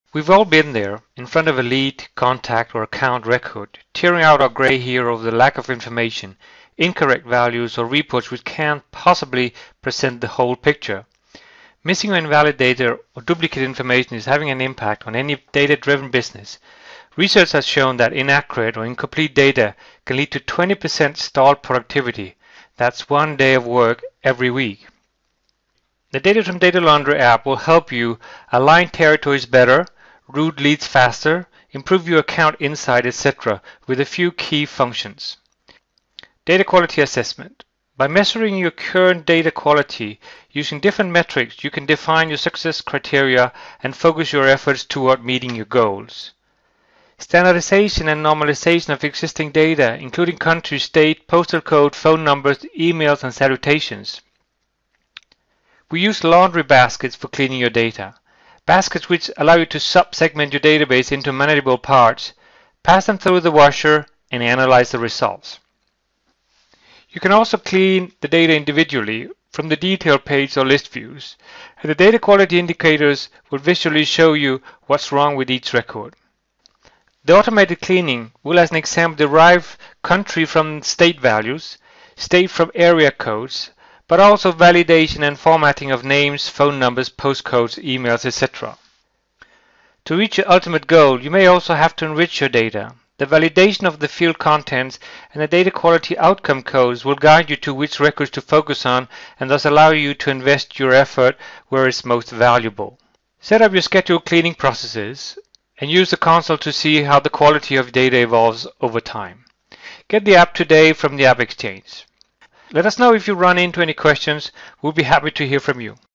DataTrim Data Laundry, Trailer
Quick introduction to the DataTrim Data Laundry App.
A Data Quality Assessment and Data Cleaning App for saleforce.